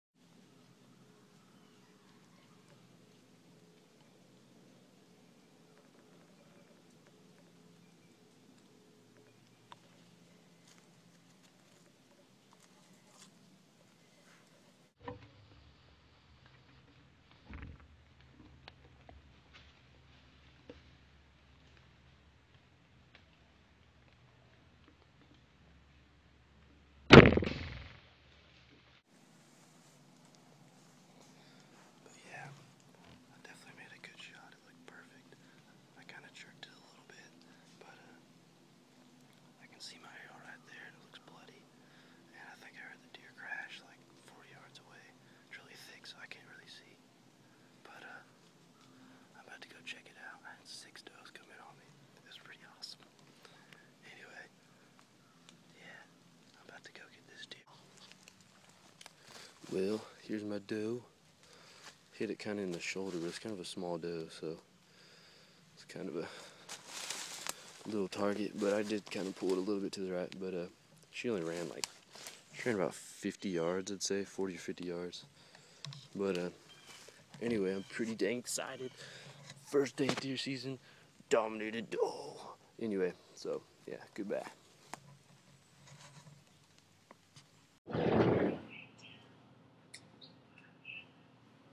Bowtech Insanity Hunt
Doe kill with my new Insanity and Rage broadhead! Used a Crazy Archery Iphone 4 mount to video. First day of deer season in east Texas! (p.s. Vid quality is terrible on a phone so try to watch on a computer)